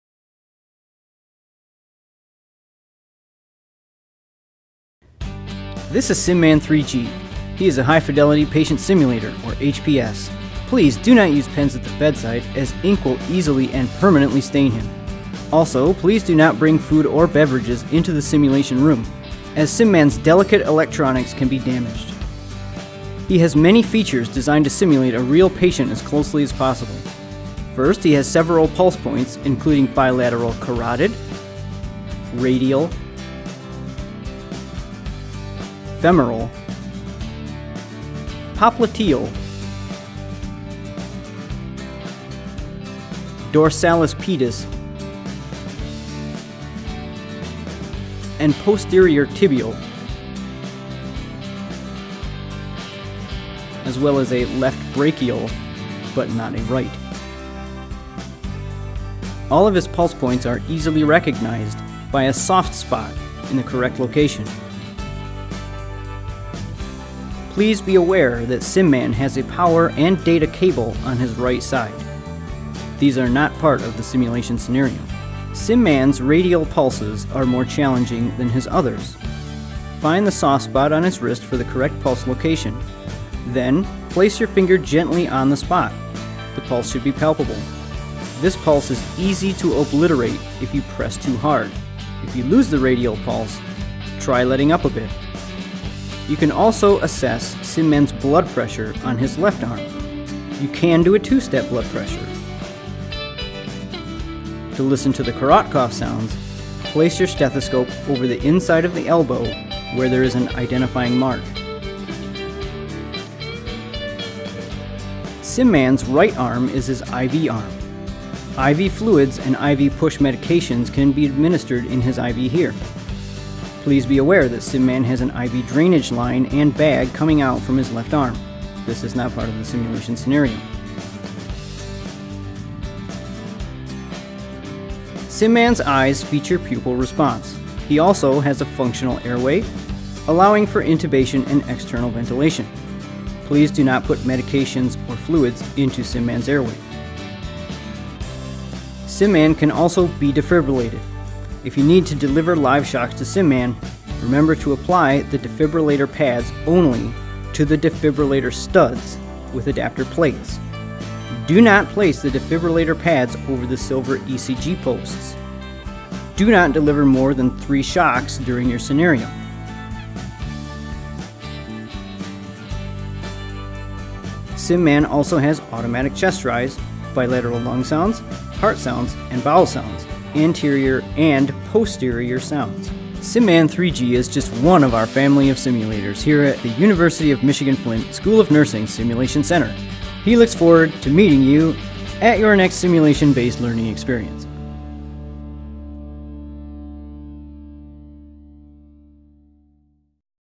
Introduction to SimMan 3G, our Human Patient Simulator
Introduction to the University of Michigan-Flint School of Nursing SimMan 3G human patient simulator